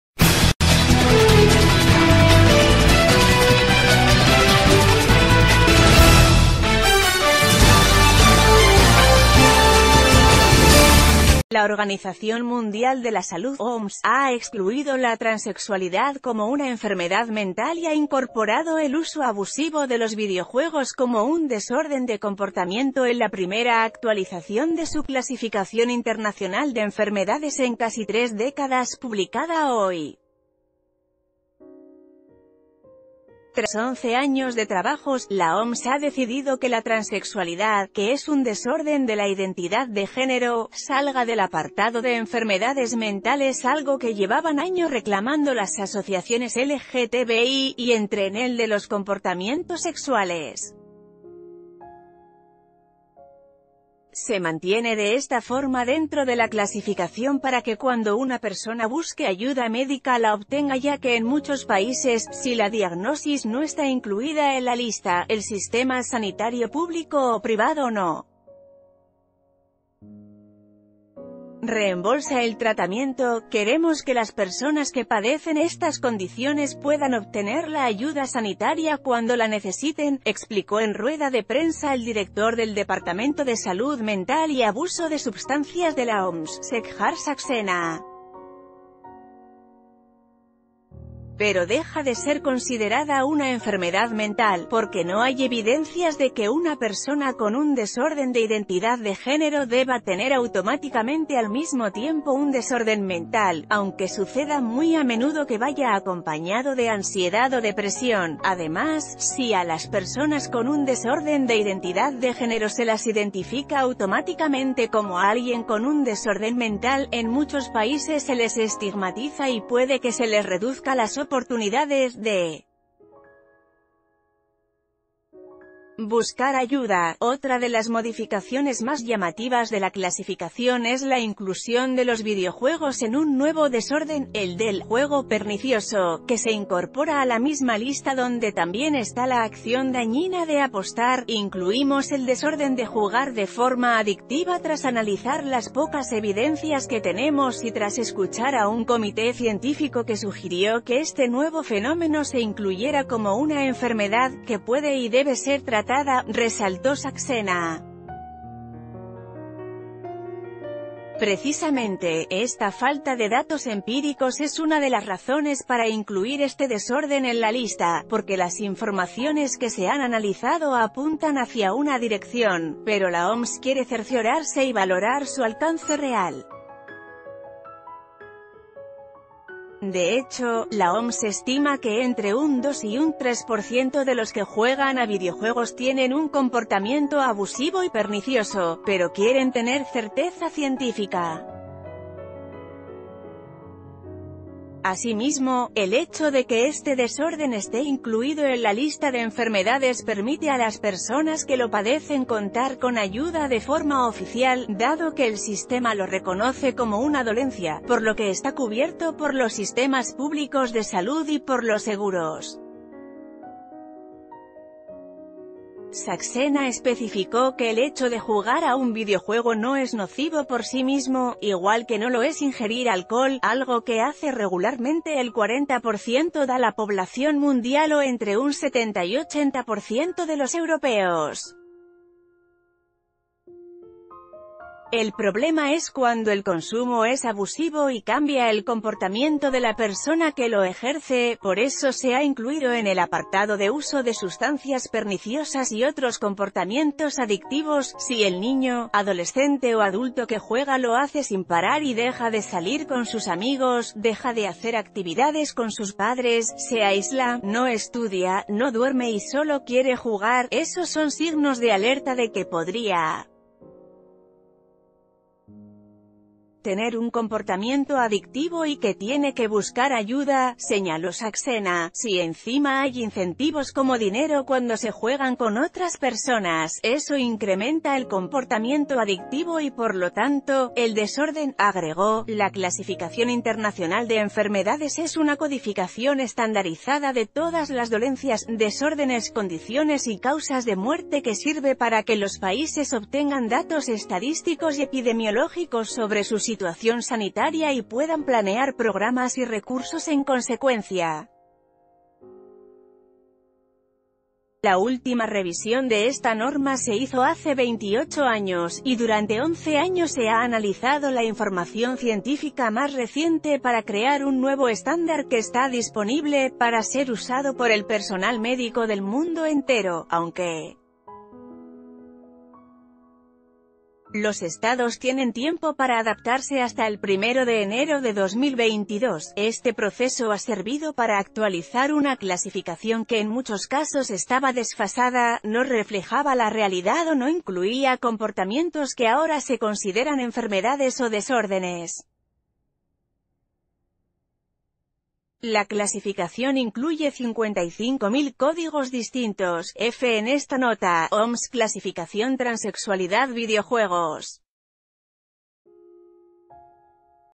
Transexualidad ya no es un trastorno mental
La Organización Mundial de la Salud (OMS) ha excluido la transexualidad como una enfermedad mental y ha incorporado el uso abusivo de los videojuegos como un desorden de comportamiento en la primera actualización de su Clasificación Internacional de Enfermedades en casi tres décadas publicada hoy. T...